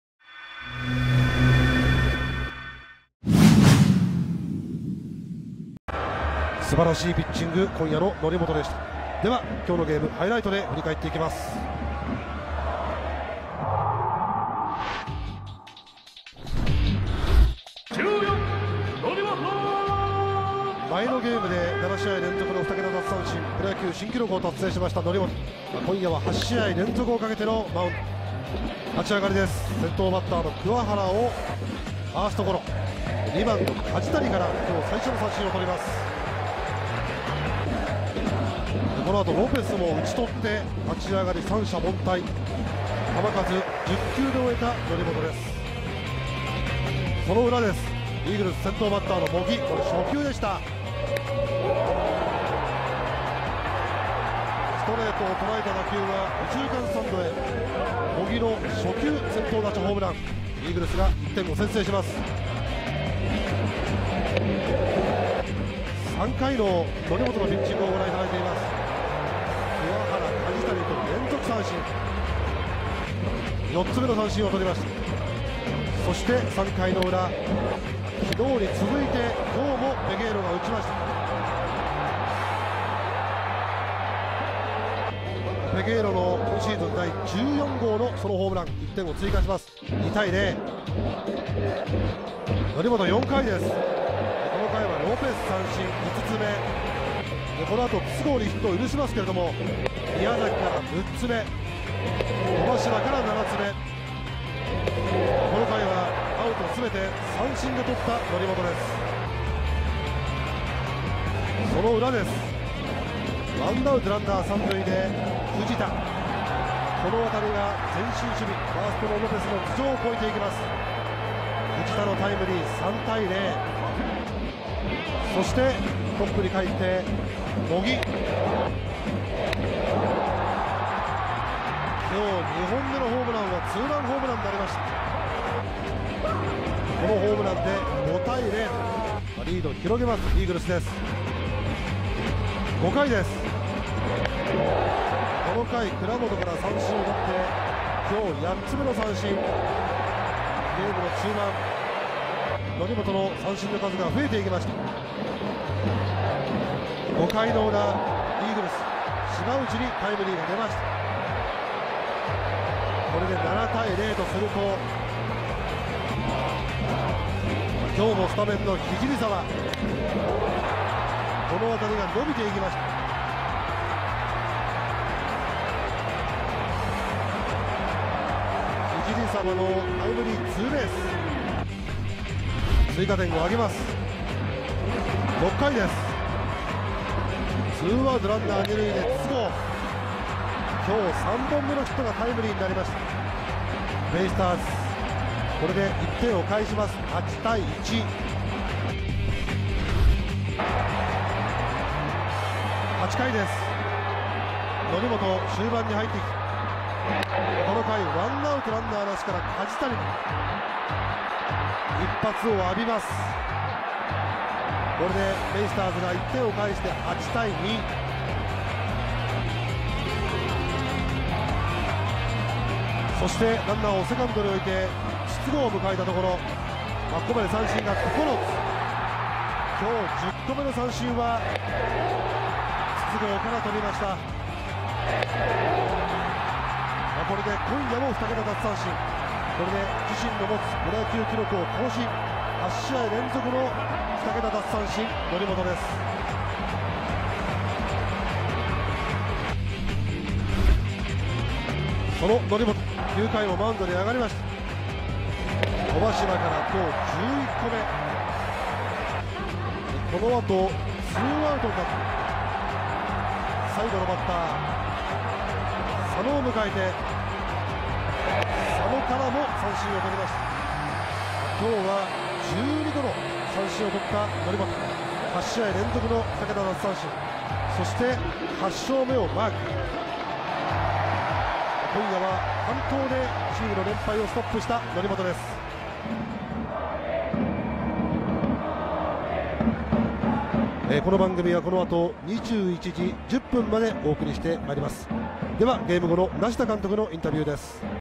2017年6月8日 楽天対横浜DeNA 試合ダイジェスト
イーグルス対ベイスターズ(Koboパーク宮城)の試合ダイジェスト動画。 2017/06/08 東北楽天ゴールデンイーグルス 対 横浜DeNAベイスターズ